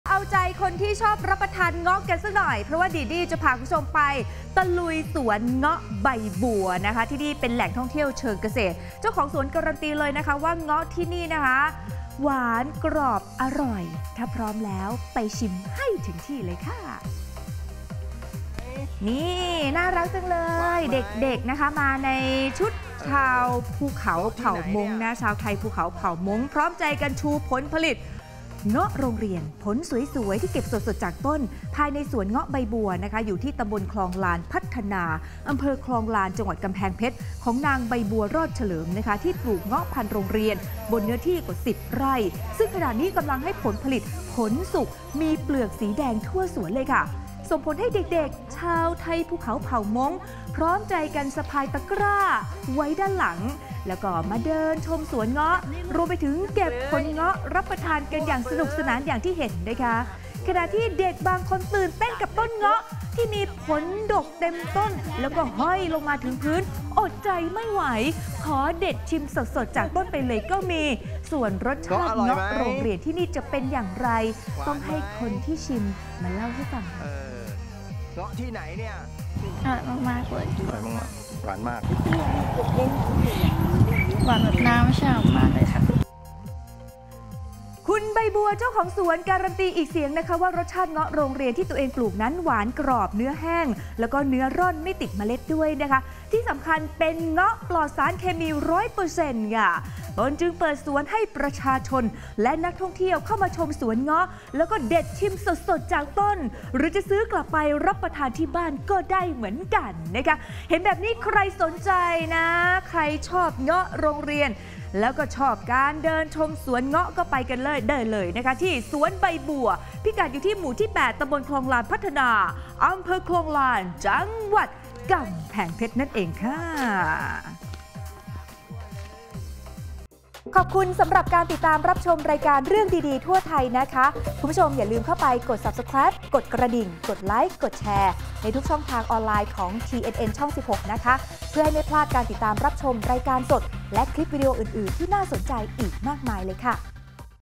สวนเงาะใบบัวกำแพงเพชร เปิดให้ชมฟรี | เรื่องดีดีทั่วไทย | 22-5-67
ตะลุย "สวนเงาะใบบัว" แหล่งท่องเที่ยวเชิงเกษตรเมืองกำแพงเพชร  ที่เจ้าของสวนการันตี รสชาติ หวาน กรอบเนื้อแห้ง แถมอร่อยมาก